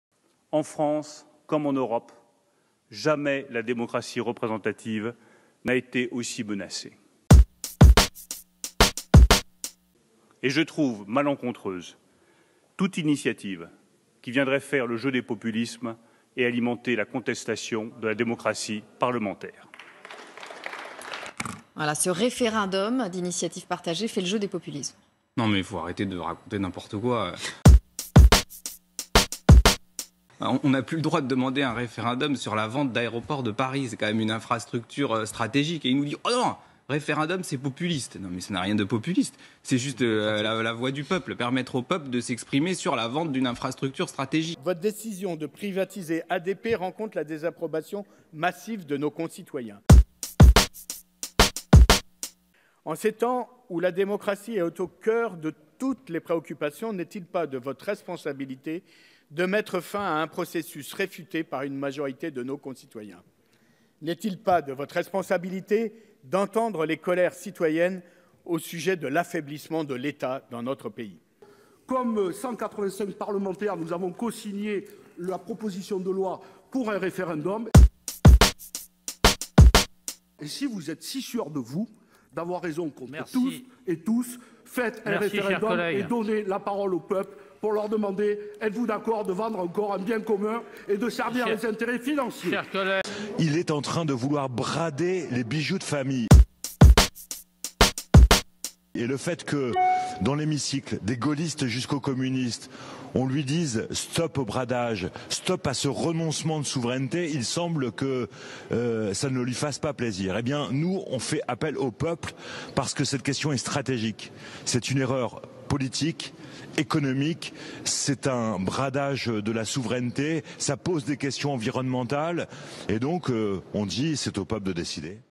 Le ministre de l’Economie nous accuse de "faire le jeu des populismes" sur la privatisation de ADP
Le ministre de l’Economie nous accuse de "faire le jeu des populismes" sur la privatisation de ADP. Réponse en vidéo de Ian Brossat et des parlementaires communistes Sébastien Jumel Fabien Gay et StéphanePeu